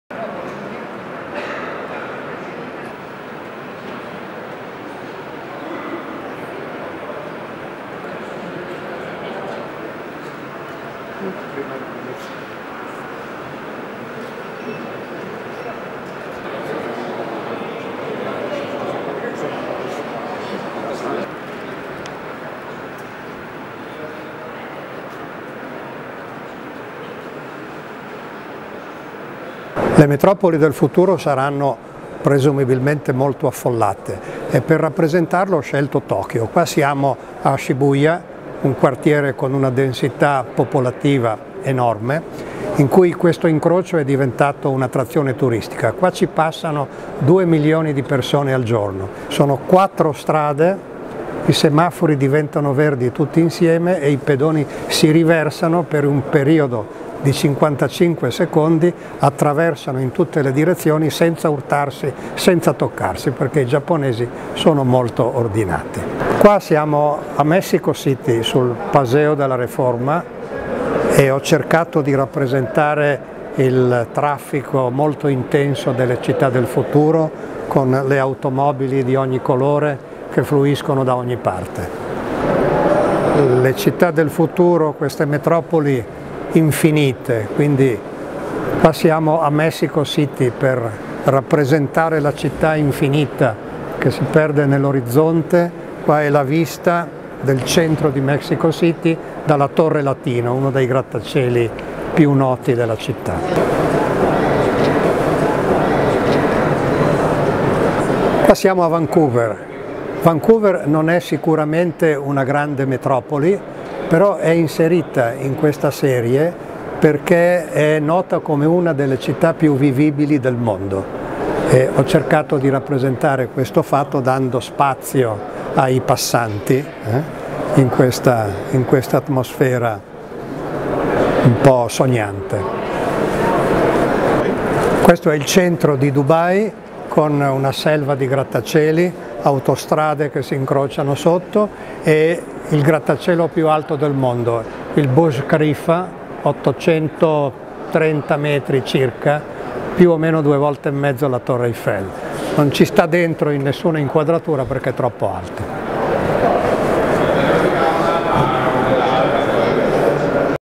2020: Noema Gallery @ BAF - Bergamo Arte Fiera, Bergamo
Partecipazione a Bergamo Arte Fiera (gennaio 2020) con alcune immagini della serie "Future & The City", presentate da Noema Gallery